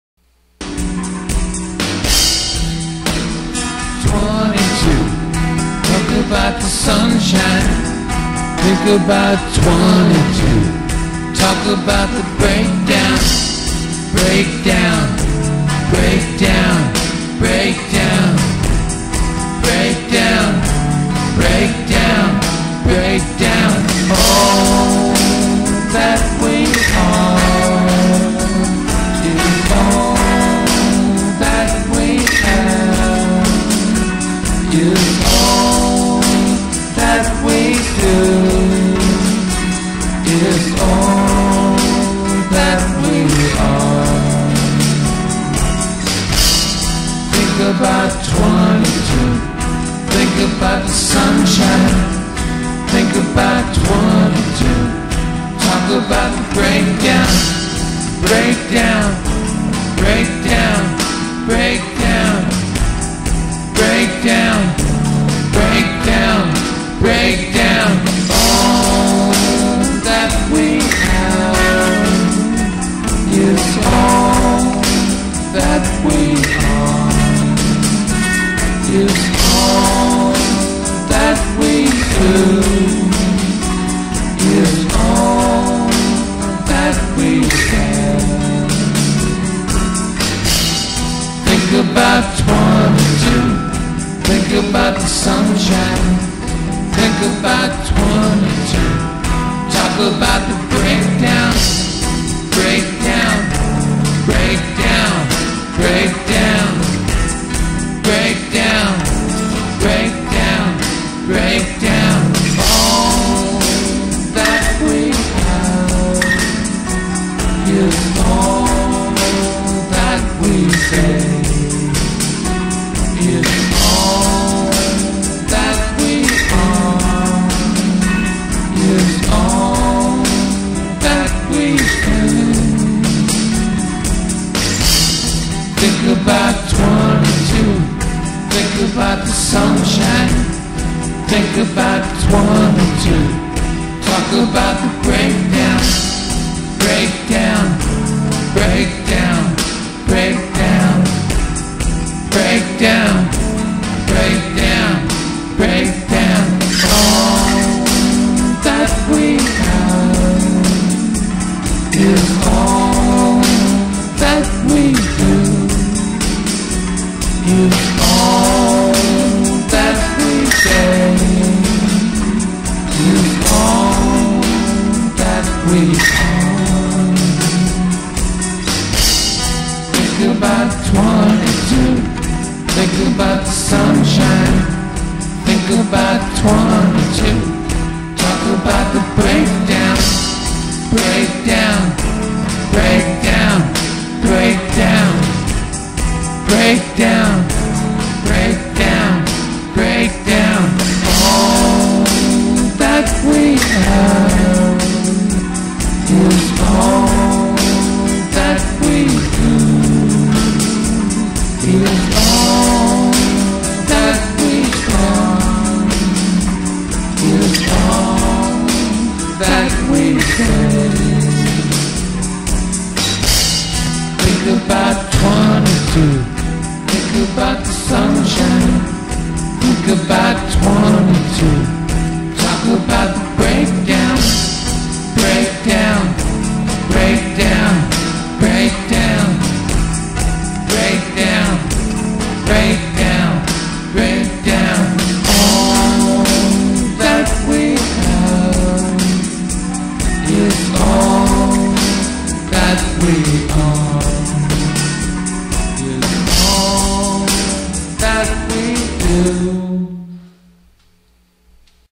about sunshine.wmv
there is a lot of nitrogen in lsd cocaine heroin and protein and cabbage and lettuce eating nitrogen isn't good and is quickly excreted as ammonia thru kidneys which fail at this over time